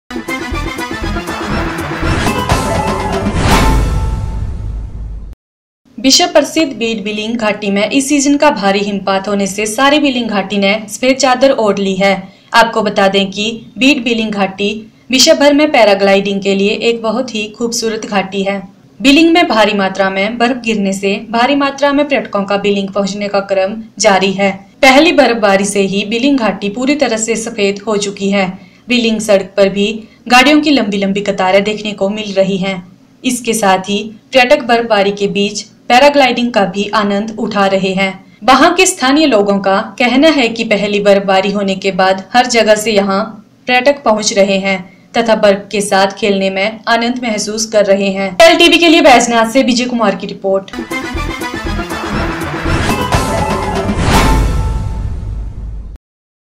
बींड बिलिंग घाटी विश्व भर में पैराग्लाइडिंग के लिए खुबसूरत घाटी
विश्व प्रसिद्ध बीड बिलिंग घाटी में इस सीजन का भारी हिमपात होने से सारी बिंलिग घाटी ने सफेद चादर ओढ ली है आपको बता दें कि बींड बिलिंग घाटी विश्व भर में पैराग्लाइडिंग के लिए एक बहुत ही खुबसूरत घाटी है